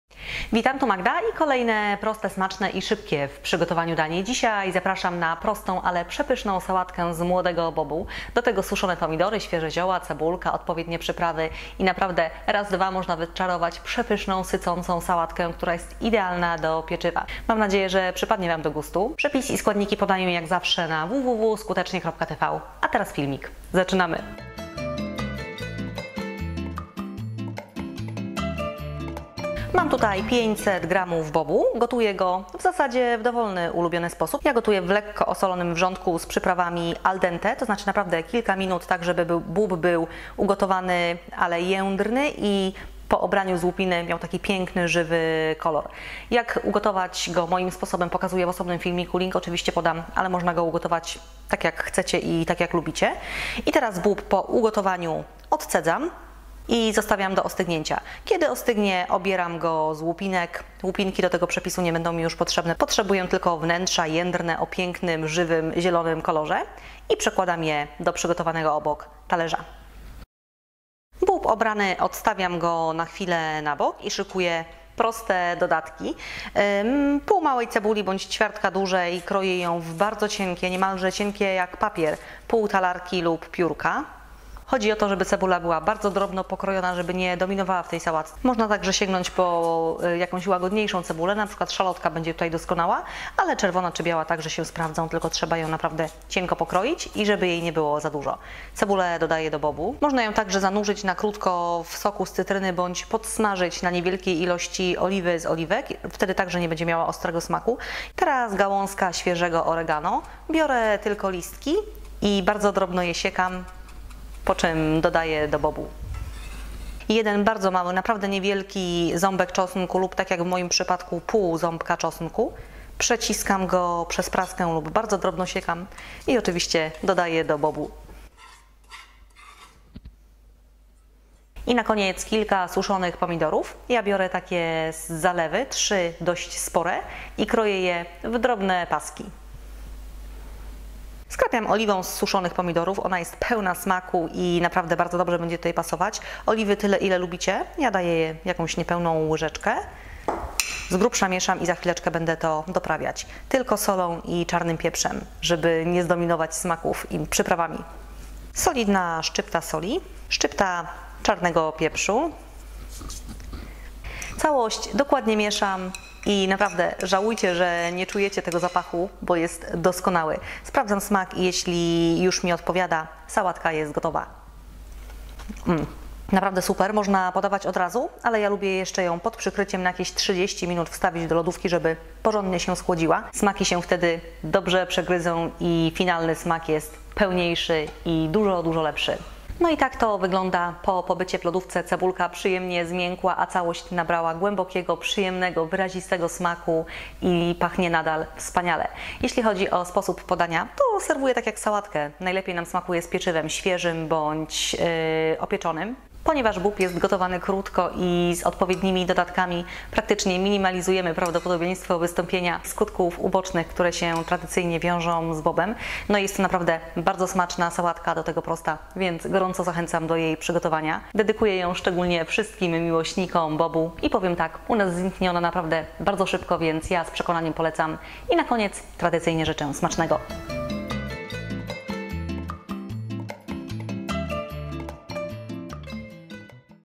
Sałatka z bobu i suszonych pomidorów :: Skutecznie.Tv [HD]
Jędrne, zielone ziarna, suszone pomidory i mnóstwo smaku, gwarantowanego przez proste, smaczne dodatki... Jednym słowem pyszna sałatka, której przygotowanie trwa przysłowiowe kilka minut. Do tego pieczywo (lub nie;)) i prosta kolacja czy przekąska ras, dwa gotowa. Uwielbiam dobrze ugotowany bób i może dlatego w tej wersji tak bardzo mi smakuje. Przepis nie jest skomplikowany, więc rozpisywać się szalenie nie będę, ale zachęcam. Wypróbujcie ten przepis, szczególnie, jeśli tak, jak ja jesteście miłośnikami bobu:)